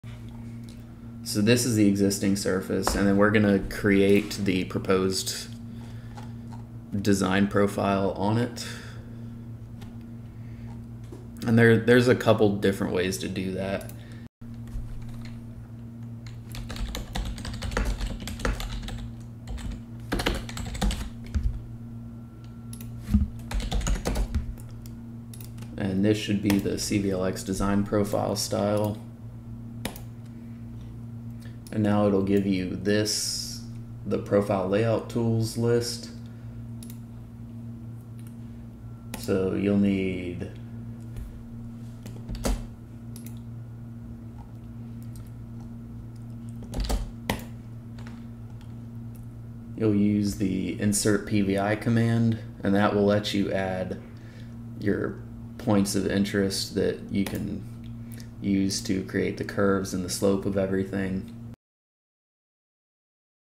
Civil 3D: Design Profile Creation Tutorial (Quick Guide)
We'll show you how to create a proposed design profile on an existing surface using Civil 3D. Use the insert PVI command to add points of interest and create curves. Learn CVLX design profile style.